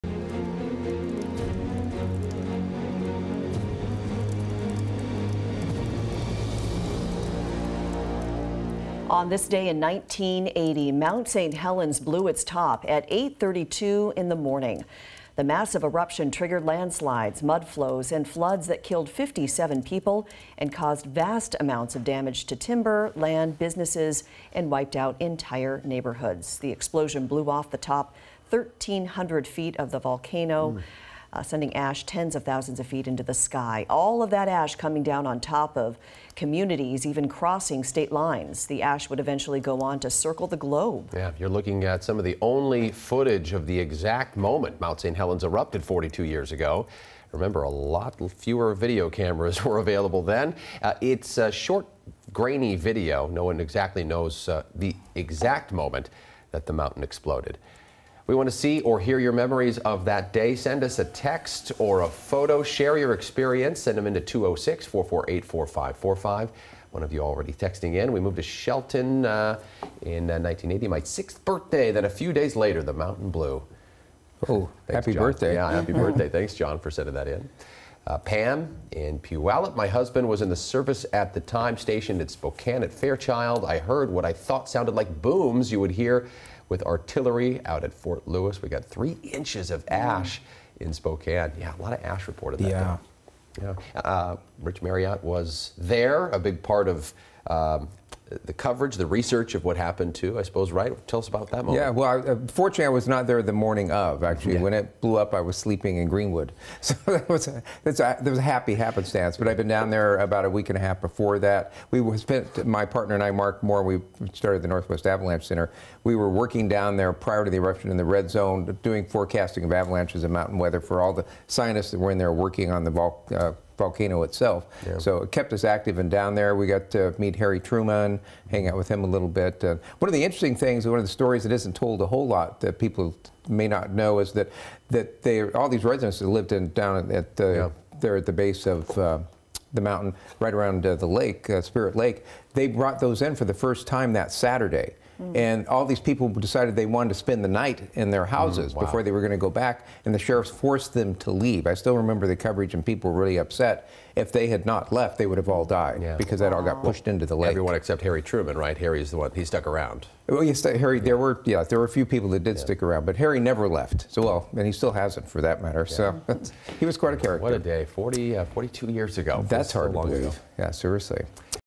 The 42nd anniversary of the Mount St. Helens eruption
On May 18, 1980, Mount St. Helens erupted, killing 57 people and decimating entire neighborhoods and nearby woodlands